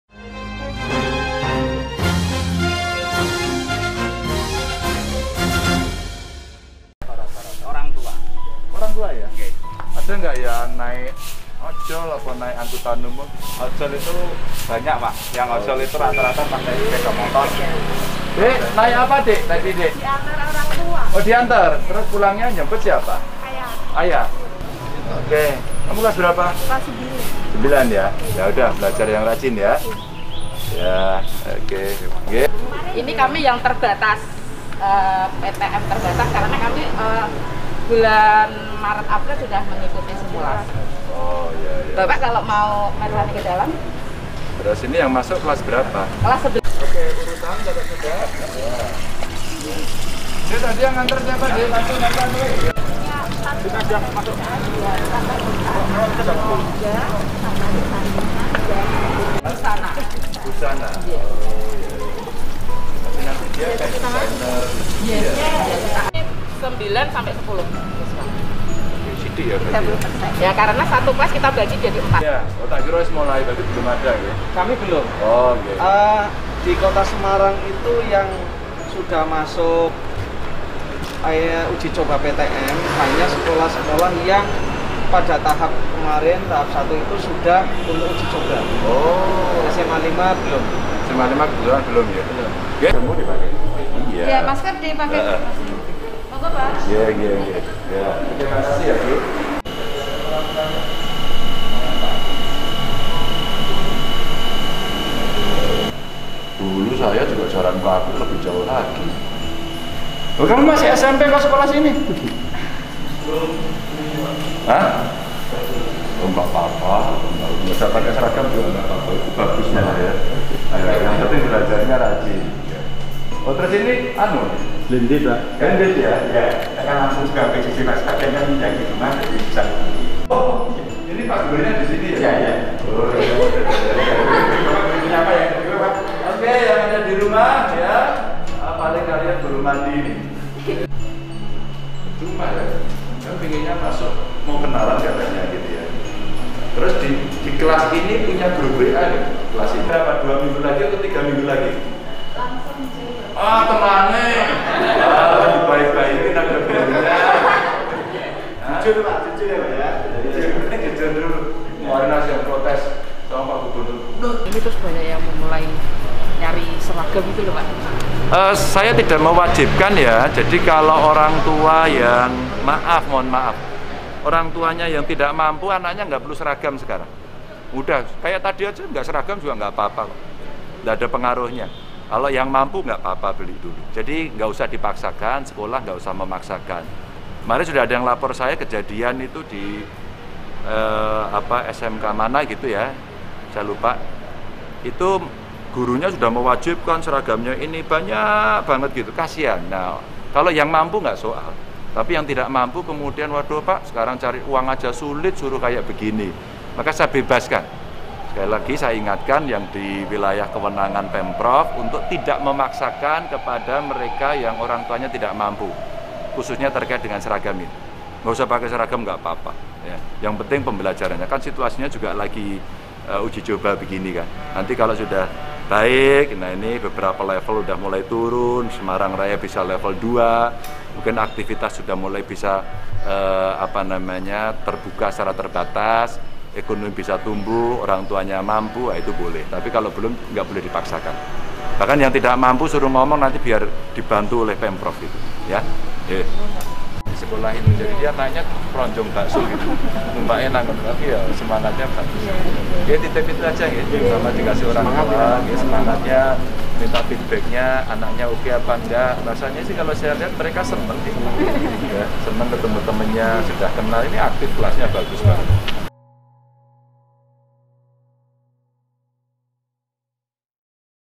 GANJAR BEBASKAN SISWA TIDAK MEMAKAI SERAGAM
Gubernur Jawa Tengah Ganjar Pranowo mengimbau kepada seluruh sekolah di bawah kewenangan Pemprov Jateng untuk tidak mewajibkan seragam. Terutama jika orangtua siswa tidak mampu.

Hal ini disampaikan Ganjar, usai gowes sambil meninjau pelaksanaan PTM di sejumlah sekolah di Kota Semarang, Selasa (31/8). Ganjar mengaku mendapat laporan terkait seragam dari salah satu orangtua siswa SMK di Jateng.

“Sekolah nggak usah memaksakan, kemarin sudah ada yang melapor ke saya kejadian itu di SMK mana saya lupa. Itu gurunya sudah mewajibkan seragamnya ini banyaaak banget gitu kasian,” katanya.

Ganjar menegaskan pihaknya tidak mewajibkan penggunaan seragam untuk siswa SMA/SMK. Sebab, hal itu tidak berpengaruh langsung pada pembelajaran. Ganjar membebaskan soal seragam ini, terutama jika orangtua siswa memang tidak mampu. Ganjar memahami dalam situasi seperti ini, tidak semua orangtua mampu untuk membeli seragam.

“Saya tidak mewajibkan ya, jadi kalau orangtua yang maaf mohon maaf, orangtuanya yang tidak mampu anaknya nggak perlu seragam sekarang. Udah kayak tadi aja nggak seragam juga nggakpapa kok. Nggak ada pengaruhnya,” ujar Ganjar.

“Kalau yang mampu, (beli seragam) nggak soal. Tapi yang tidak mampu kemudian ‘waduh pak sekarang cari uang aja sulit suruh kayak begini’. Maka saya bebaskan,” ujarnya.

“Sekali lagi saya ingatkan, (sekolah) yang di wilayah kewenangan Pemprov untuk tidak memaksakan kepada mereka yang orang tuanya tidak mampu khususnya terkait dengan seragam,” tegas Ganjar.

Ganjar mengatakan, pelaksanaan PTM saat ini harus dimanfaatkan agar siswa bisa belajar dengan nyaman dan orangtua juga tenang. Soal seragam, kata Ganjar, tak harus sama karena tidak berpengaruh pada pembelajaran.

“Nggak usah pakai seragam nggakpapa, yang penting pembelajarannya. Kan situasinya juga lagi uji coba begini kan,” katanya.

Jika nanti kondisinya semakin baik, lanjut Ganjar, apalagi beberapa daerah mulai turun level PPKMnya, ekonomi mulai bergerak dan orangtuanya mampu maka dibolehkan.

“Nanti kalau sudah baik, beberapa level udah mulai turun. Semarang raya udah level dua. Mungkin aktivitas sudah mulai bisa terbuka secara terbatas, ekonomi bisa tumbuh orangtuanya mampu itu boleh,” ujarnya

Ganjar mengungkapkan, jika ada orangtua siswa yang tidak mampu bisa melapor kepada Pemprov Jateng agar dibantu.

“Tapi kalau belum nggak boleh dipaksakan. Bahkan yang tidak mampu suruh ngomong nanti biar dibantu oleh Pemprov,” tandasnya.